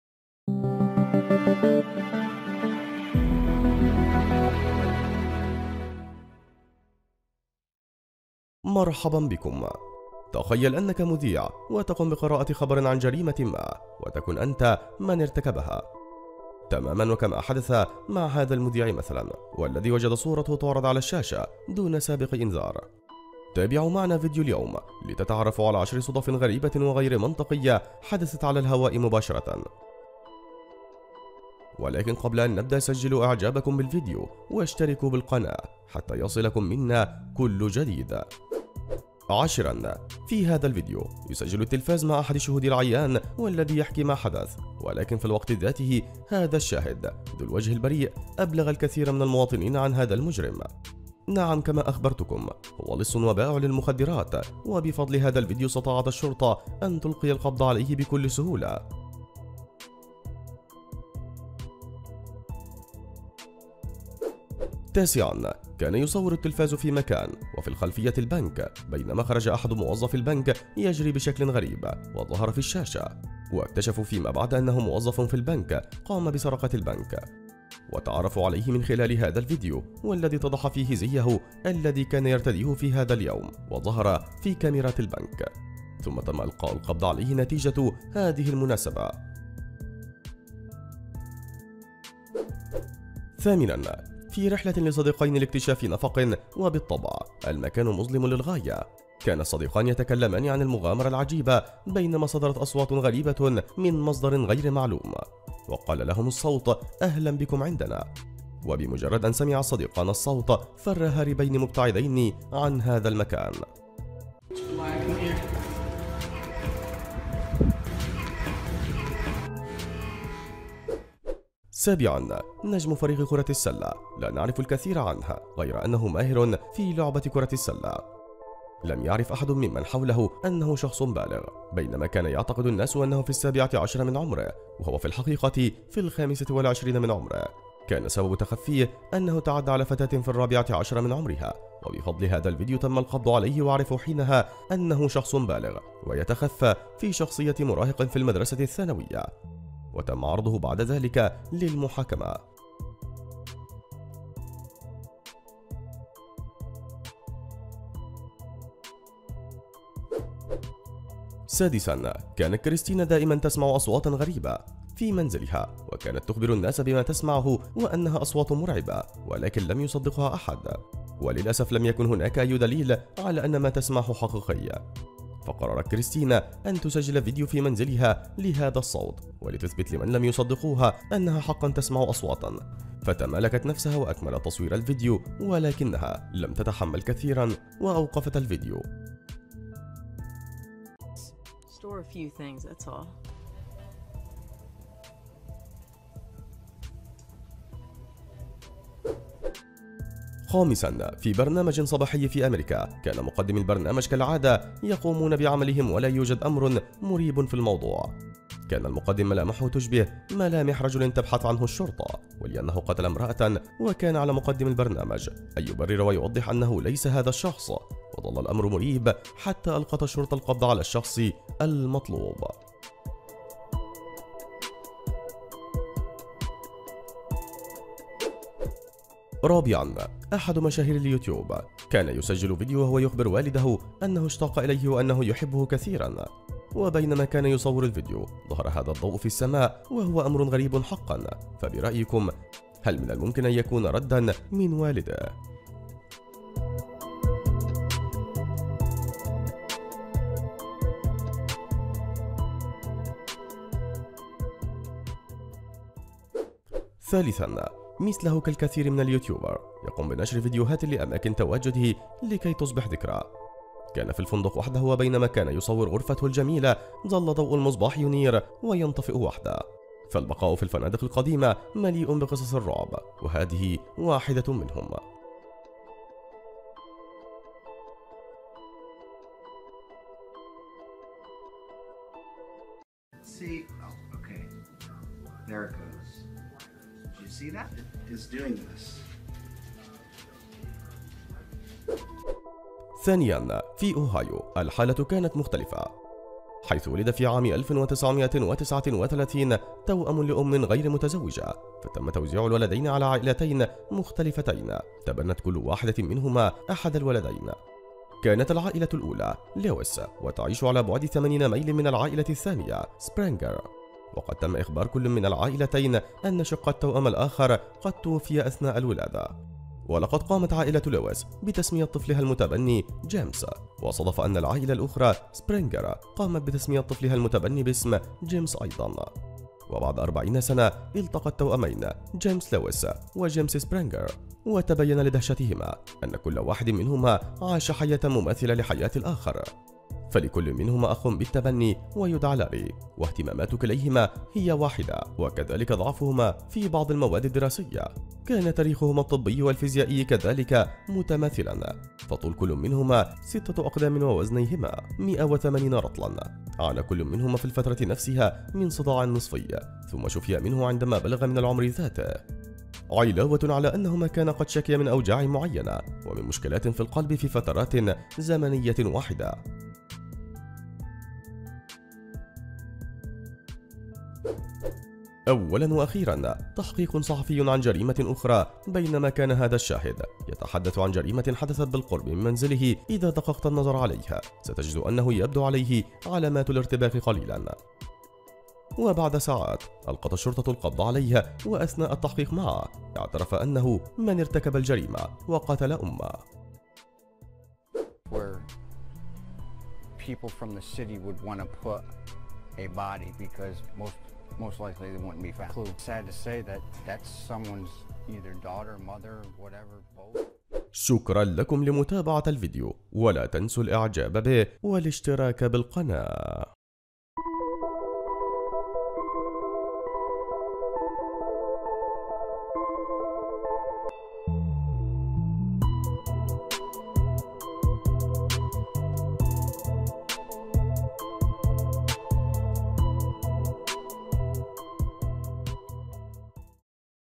10 صدف غريبة وغير منطقية حدثت على الهواء مباشرة !!
10 صدف غريبة وغير منطقية حدثت على الهواء مباشرة .. تخيل أنك مذيع وتقوم بقراءة خبر عن جريمة ما وتكن أنت من ارتكبها! كما حدث مع هذا المذيع مثلاً، الذي وجد صورته تعرض على الشاشة دون سابق إنذار، تابعوا معنا فيديو اليوم لتتعرفوا على 10 صدف غريبة وغير منطقية حدثت على الهواء مباشرة.